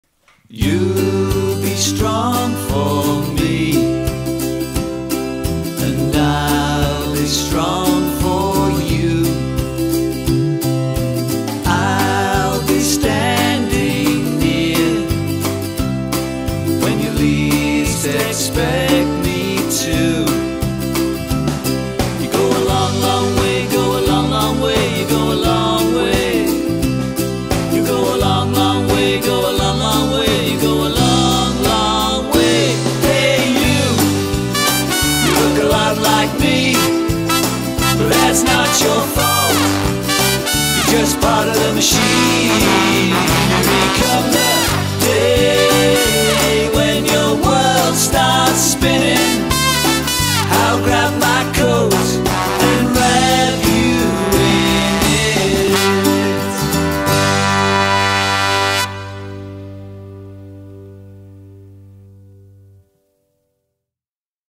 Video Clips - Summer 2023 - musical duo the Pelican Babies
The Pelican Babies songwriting duo feature on this short compilation video with a shortened version of a new song from their next album - title to be decided!!! It's a round-up from Summer of 2023 when they performed their self-penned songs on guitar, harmonica and of course didgeridoo in their own close harmony style. They were joined by percussionist Pete West on some of their gigs over the summer and Lea and Mark would like to officially thank him for his superb contribution.